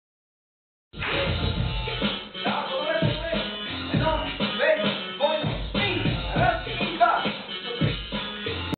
HIP-HOP classes 001 on 19.I.2011 @ Salsa Ritmo Dance Studio, Sofia
Хип-хоп с Коста в залата на Salsa Ritmo - Студентски град